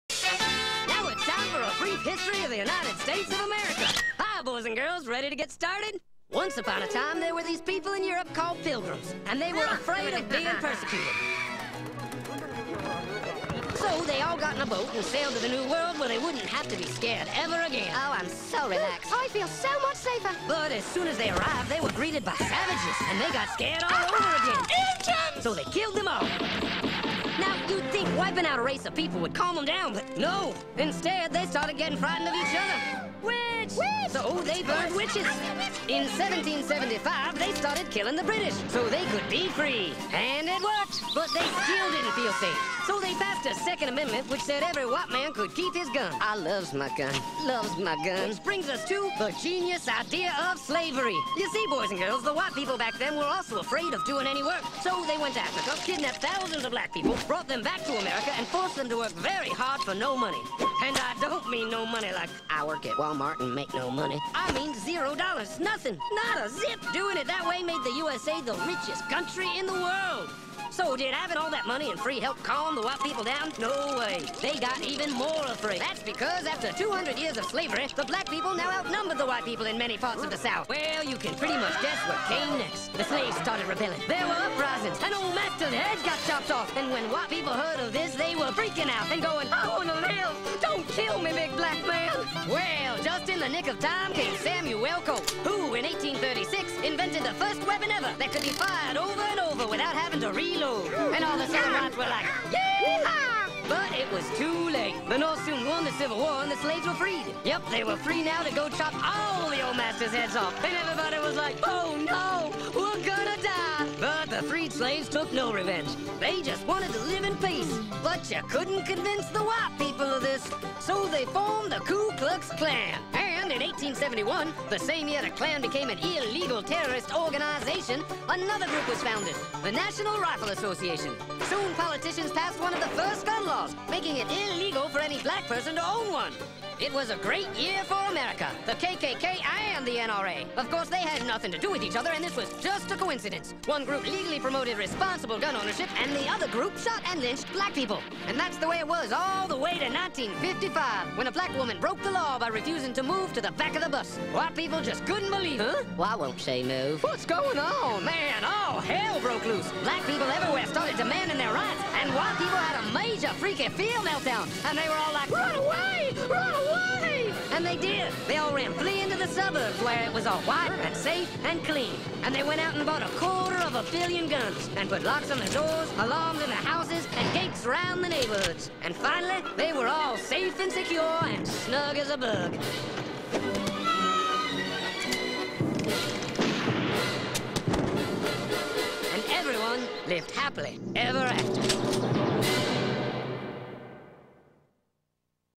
Conflict Theory as Applied to American History
The historical accuracy of the video is based on interpretation and definitely not chronological.  5 min animation about fear and guns in U.S. of A.